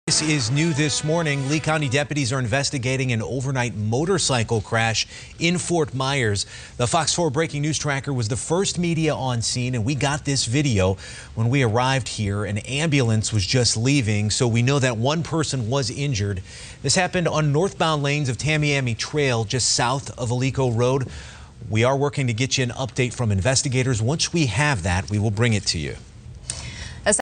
Injury reported in overnight motorcycle crash on Tamiami Trail near Alico Road
This year’s races are on November 9th and 10th.  The crash happened just before 3:00 a.m. about a quarter of a mile South of Alico Road in the northbound lanes